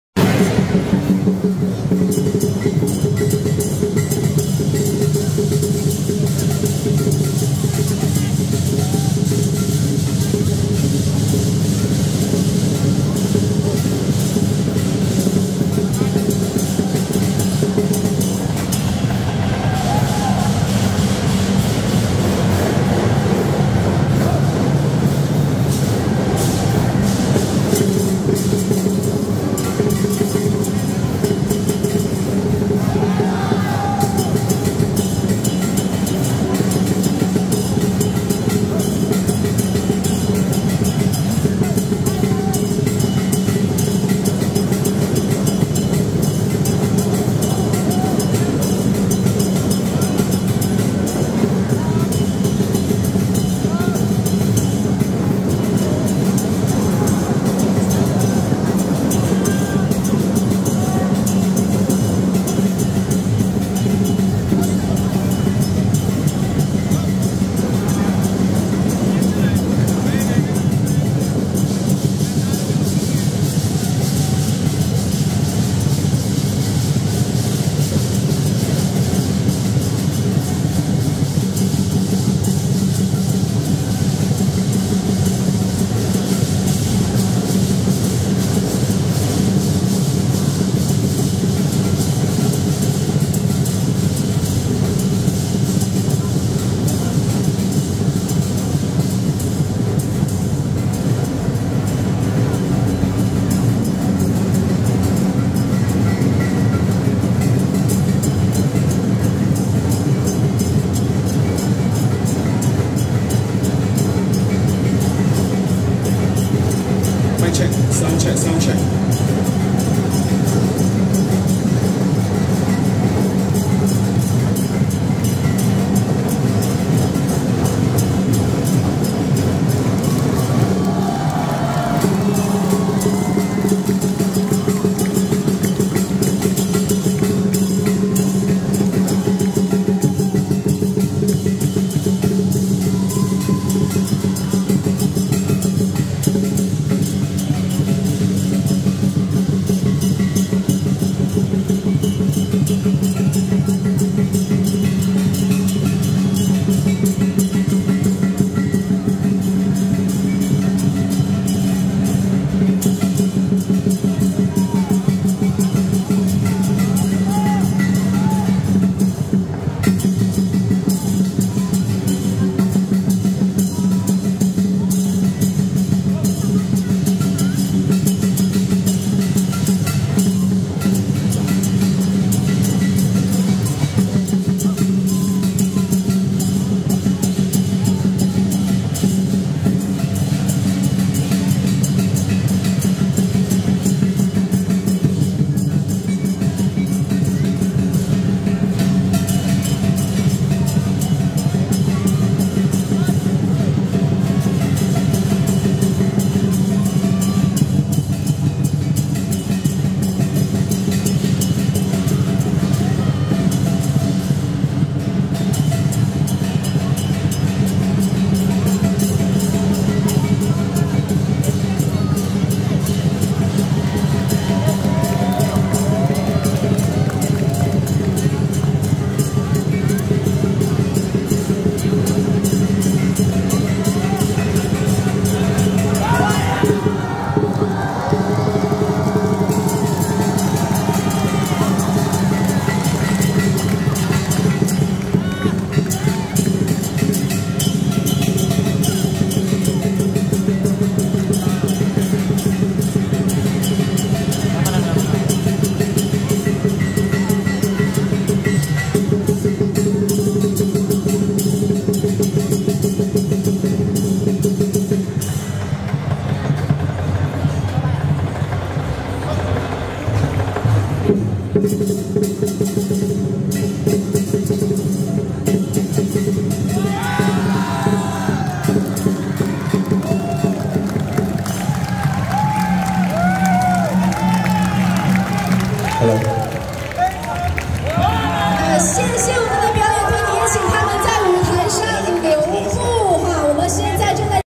2023 馬來西亞龍獅節 WDLDE / Grand Finale - Dragon Dance by 吉隆坡新洋 KL Xin Yang
2023 馬來西亞龍獅節 World Dragon & Lion Dance Extravaganza - Malaysia Edition

龍獅巡遊鬧元宵 CHAP GOH MEI Grand Finale Parade

CNY Day 15 初十五

Dragon dance performance by Malaysia Kuala Lumpur Xinyang Athletic Association 马来西亚吉隆坡新洋体育会 at LaLaport BUKIT BINTANG CITY CENTRE.

5 February 2023